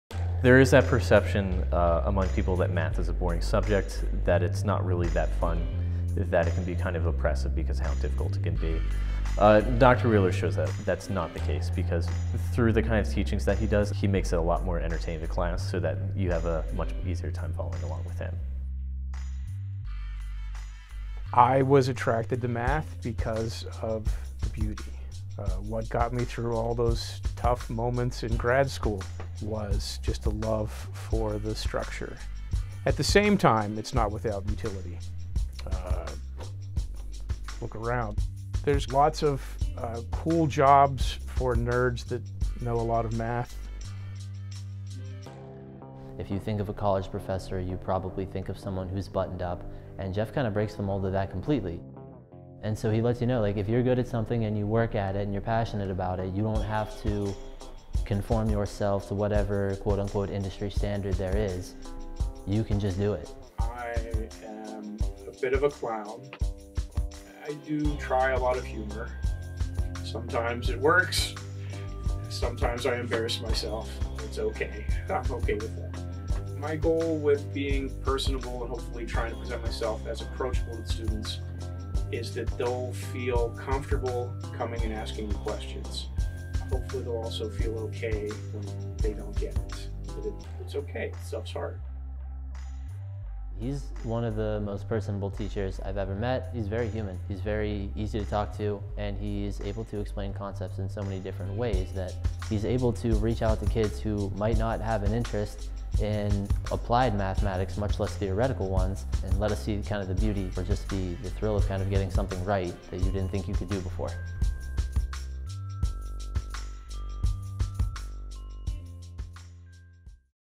Classroom Sneak Peek: Mathematics
Find out what it's like to study mathematics with Lecturer Jeffrey Wheeler.